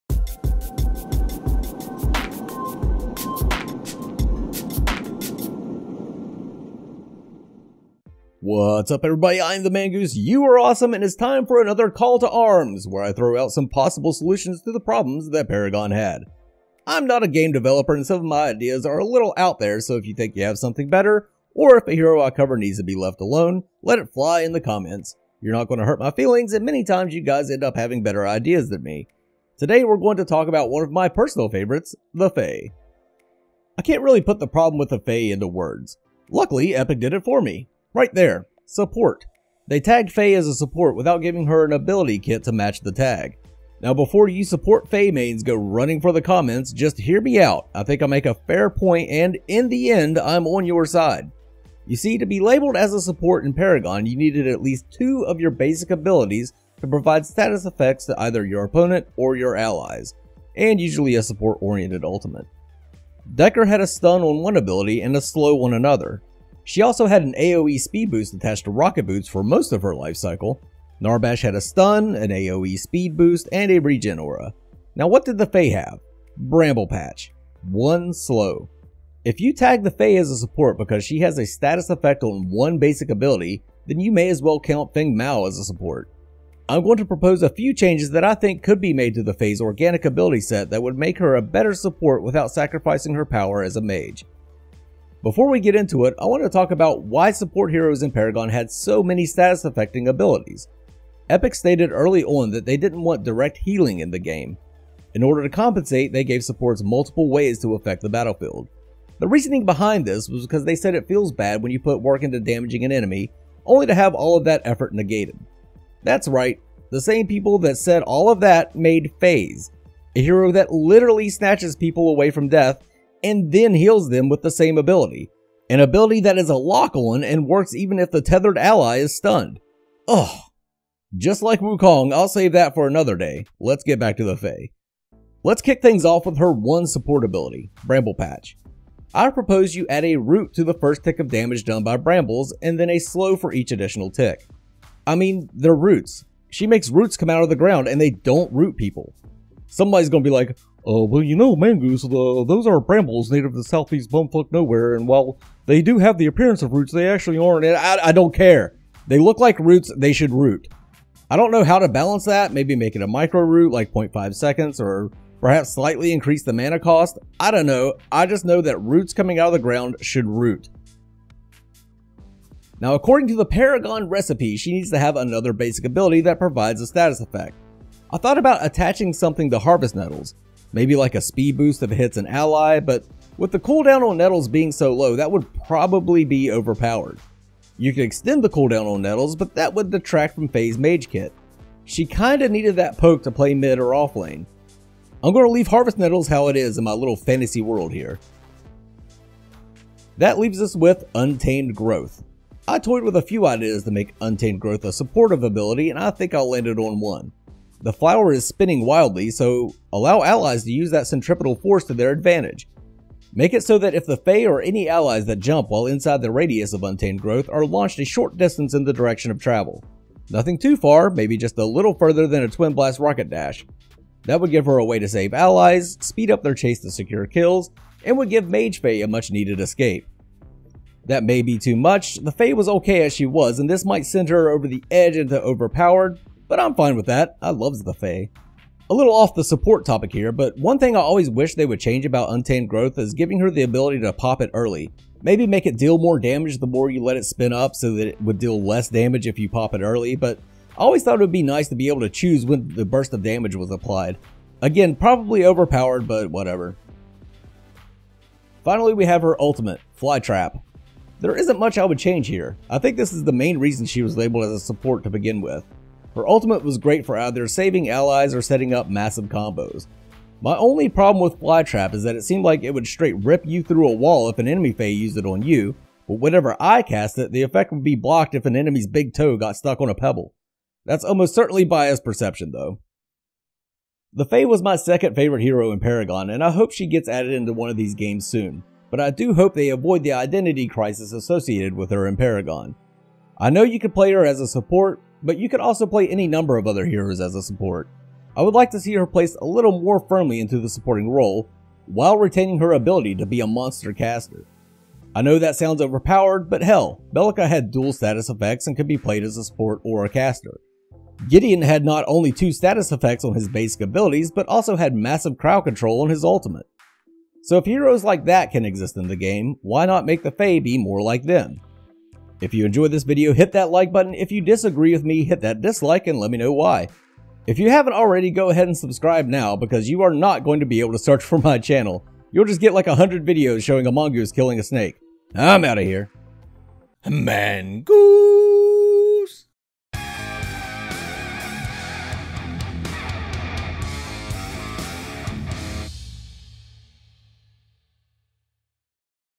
Call to Arms: Was The Fey a Support?
Epic retained the support/guardian tag for The Fey without ever providing her with the necessary kit to back it up. I have a problem with that!